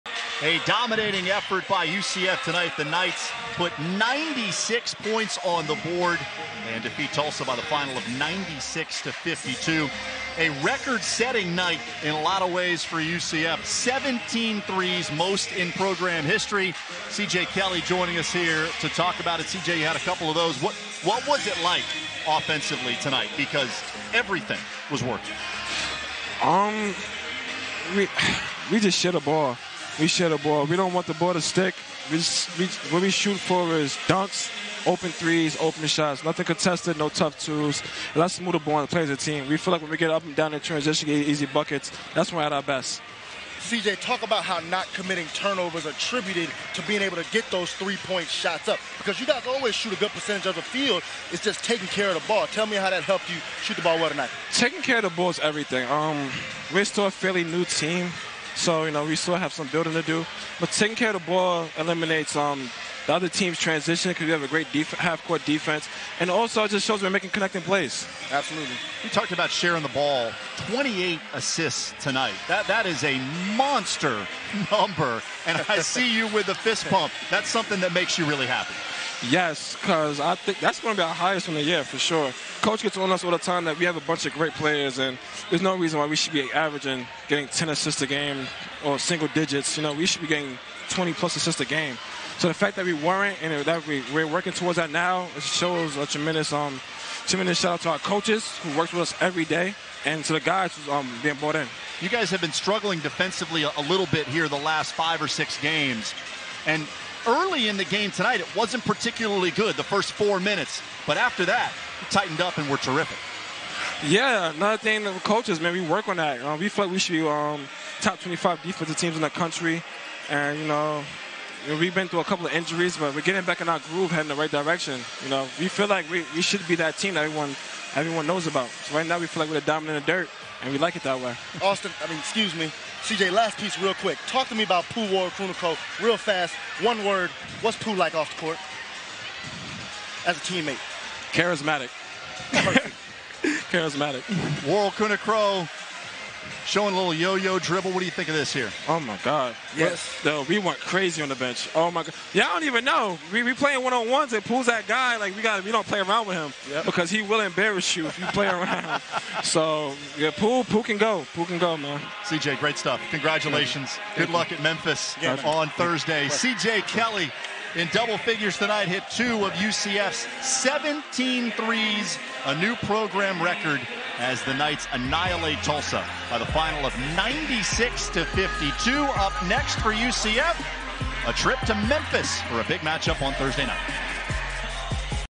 ESPN Recap: UCF 96, Tulsa 52
The UCF men's basketball team, going by the nickname Citronauts for the evening's Space Game, defeated Tulsa by 44 points on a night which saw the Knights bury a program record 17 three pointers. C.J. Kelly stopped by to chat with Austin Lyon and B.J. Taylor to discuss the victory.